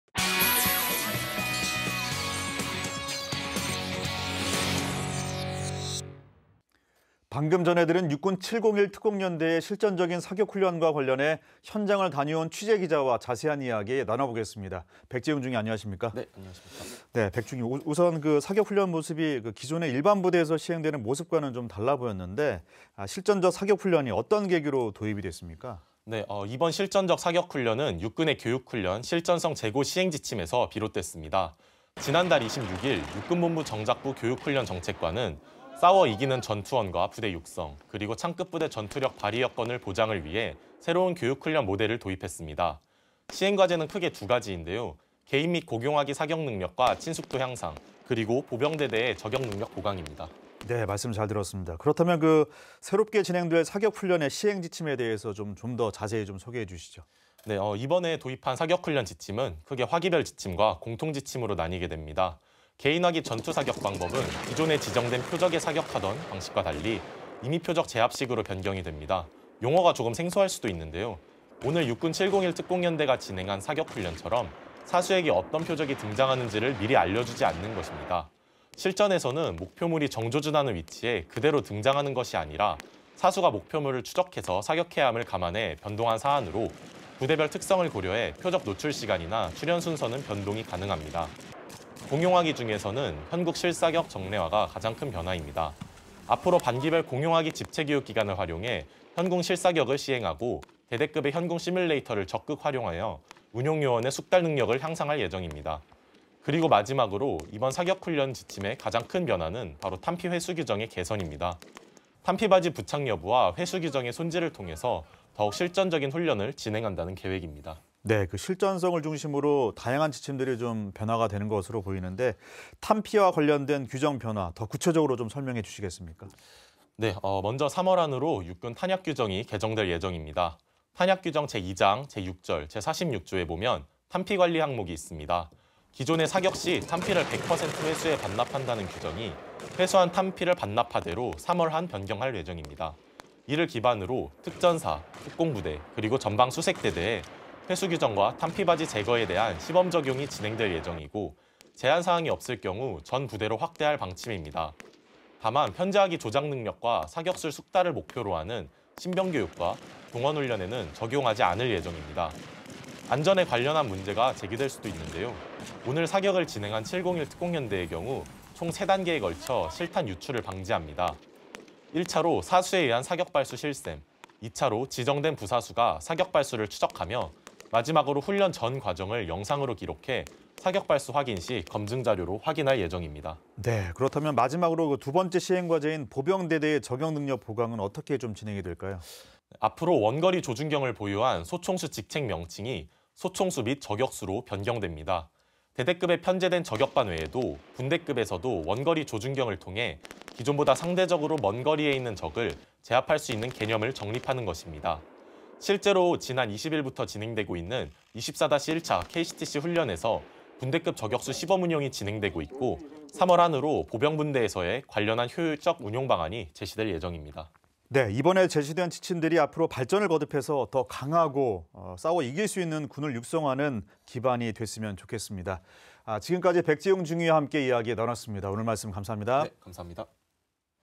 [기자출연] ‘창끝 전투력’…육군 교육훈련 실전성 제고 [국방홍보원]
방금 보도한 실전적인 사격훈련과 관련해 현장을 다녀온 취재기자와 더 자세한 이야기 나눠보겠습니다.

백재웅중위 안녕하십니까.

[기자]
: 네 안녕하십니까.

1. 오늘 사격훈련 모습이 기존에 일반 부대에서 시행되는 모습과는 사뭇 달라 보였는데요, 이 실전적 사격훈련이 어떤 계기로 도입된 것입니까?

[기자]
: 이번 실전적 사격훈련은 육군의 교육훈련 ‘실전성 제고’ 시행지침에서 비롯됐습니다.

지난달 26일, 육군본부 정작부 교육훈련정책과는 적과 싸워 이기는 전투원과 부대 육성, 창끝부대 전투력 발휘 여건 보장을 위한 새로운 교육훈련 모델을 도입했습니다.

시행과제는 크게 두 가지인데요, 개인 및 공용화기 사격능력과 친숙도 향상, 그리고 보병대대의 저격능력 보강입니다.

2. 예 그렇군요, 그렇다면 새롭게 진행될 사격훈련의 시행 지침을 알 수 있을까요?

[기자]
: 이번에 도입된 사격훈련 지침은 크게 화기별 지침과 공통 지침으로 나뉩니다.

개인화기 전투사격 방법은 기존에 지정된 표적에 사격하던 방식과 달리, 임의표적 제압식으로 변경됩니다.

용어가 조금 생소할 수도 있는데요, 오늘 육군 701특공연대가 진행한 사격훈련처럼 사수에게 어떤 표적이 등장하는지를 미리 알려주지 않는 것입니다.

실전에서는 목표물이 정조준하는 위치에 그대로 등장하는 것이 아니라, 사수가 목표물을 추적해서 사격해야 함을 감안해 변동된 사안으로, 부대별 특성을 고려해 표적 노출시간이나 출현 순서는 변동이 가능합니다.

공용화기 중에서는 현궁 실사격 정례화가 큰 변화입니다.

앞으로 반기별 공용화기 집체교육 기간을 활용해 현궁 실사격을 시행하고, 대대급의 현궁 시뮬레이터를 적극 활용해 운용요원의 숙달능력을 향상할 예정입니다.

그리고 마지막으로, 이번 사격훈련 지침의 가장 큰 변화는 바로 탄피 회수규정의 개선입니다.

탄피받이 부착 여부와 회수 규정의 손질을 통해 더욱 실전적인 훈련을 진행한다는 계획입니다.

3. 탄피와 관련한 규정변화, 더 구체적으로 알 수 있겠습니까?

[기자]
: 먼저 3월 한으로, 육군 탄약 규정이 개정될 예정입니다.

탄약규정 제2장 제6절 제46조에 탄피관리 항목이 있습니다.

기존에 사격 시 탄피를 100% 회수해 반납한다는 규정이 ‘회수한 탄피를 반납하되’로 3월 한 변경될 예정입니다.

이를 기반으로 특전사, 특공부대, 그리고 전방 수색대대에 회수규정과 탄피받이 제거에 대한 시범적용이 진행될 예정이고, 제한사항이 없을 경우 전 부대로 확대할 방침입니다.

다만 편제화기 조작능력과 사격술 숙달을 목표로 하는 신병교육과 동원훈련에는 적용하지 않을 예정입니다.

안전에 관련한 문제가 제기될 수도 있는데요, 오늘 사격을 진행한 701특공연대의 경우 총 세 단계에 걸쳐 실탄 유출을 방지합니다.

1차로 사수에 의한 사격발수 실셈, 2차로 지정된 부사수가 사격발수를 추적하며, 마지막으로 훈련 전 과정을 영상으로 기록해 사격 발수 확인 시 검증자료로 확인합니다.

4. 네 그렇다면 마지막으로, 두 번째 시행과제인 보병대대의 저격능력 보강은 어떻게 진행됩니까?

[기자]
: 앞으로 원거리 조준경을 보유한 소총수 직책명칭이 소총수 및 저격수로 변경됩니다.

대대급에 편제된 저격반 외에도, 분대급에서도 원거리 조준경을 통해 기존보다 상대적으로 먼 거리에 있는 적을 제압할 수 있는 개념을 정립하는 것입니다.

실제로 지난 20일부터 진행되고 있는 24-1차 KCTC 훈련에서 분대급 저격수 시범운용이 진행되고 있고, 3월 한으로 보병분대에서의 관련한 효율적 운용방안이 제시될 예정입니다.

이번에 제시된 지침들이 앞으로 발전을 거듭해 더 강하고, 싸워 이길 수 있는 군이 만들어지기를 바랍니다.

지금까지 백재웅중위 와 함께 이야기 나눴습니다.

오늘 말씀 감사합니다.